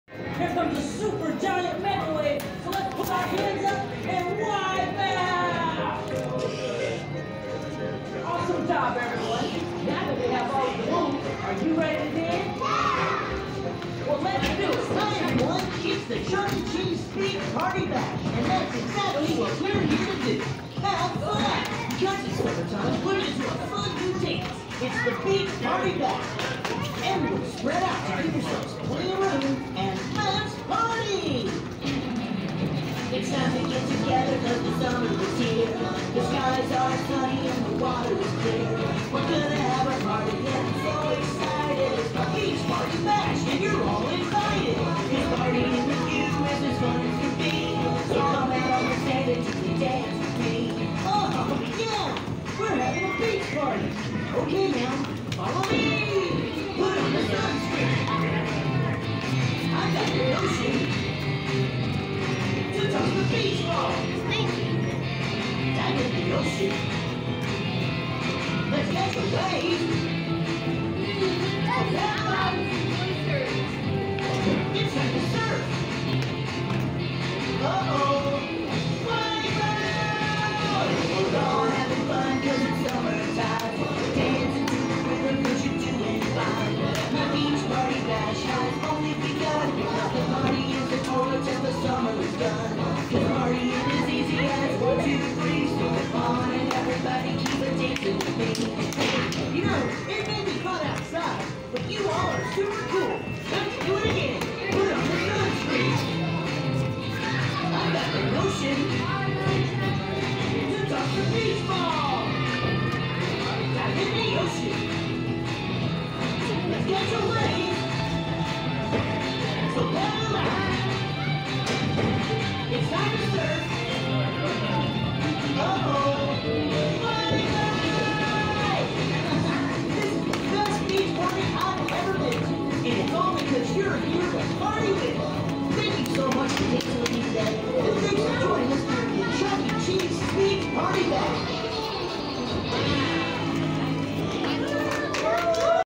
"Beach Party Bash" Chuck E Cheese Chandler AZ live show
I got permission to film the kids in this video, I came in with them. Filmed 6/27/23